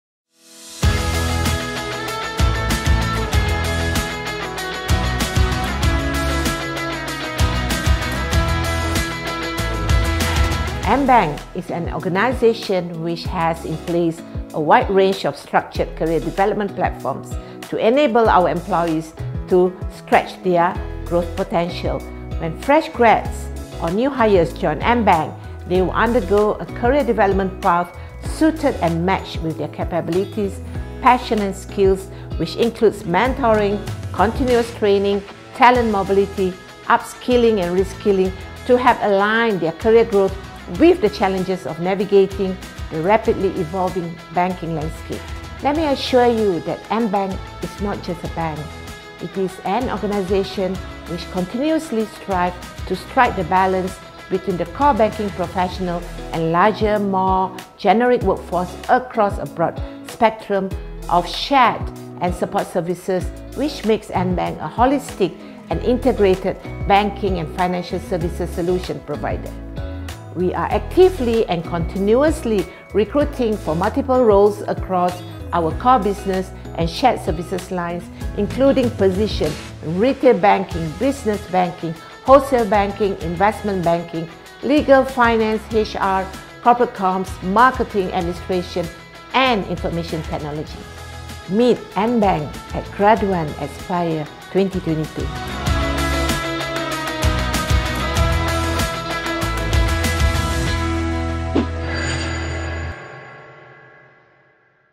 Meet AmBank Group at GRADUAN Aspire 2022!
Kick-start your career with Malaysia's leading financial services group, AmBank!

Allign your career goals and discover limitless opportunities for your career advancement with Ambank this 30 July - 2 August only

Mark your calendar and see you there!